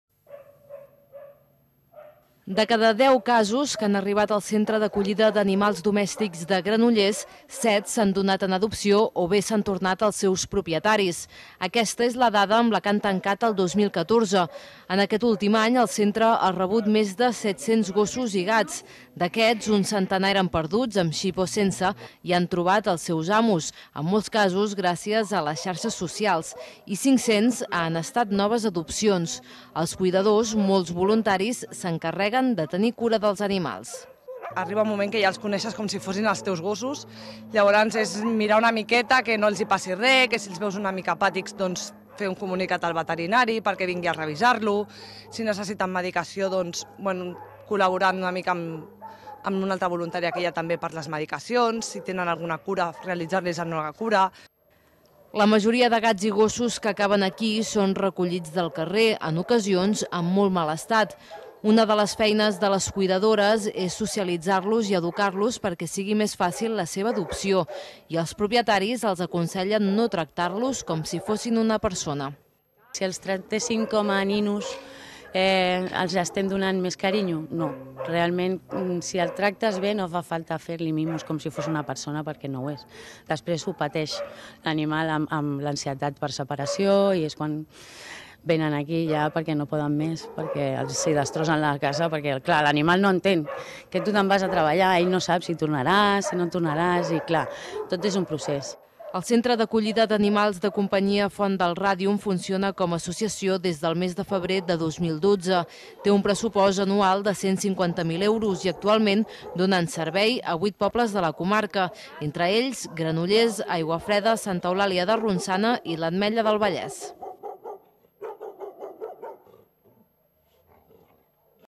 VOTV - Més de 500 gats i gossos adoptats durant el 2014 a Granollers
Noticia emesa a l'informatiu del día 27 de gener de 2015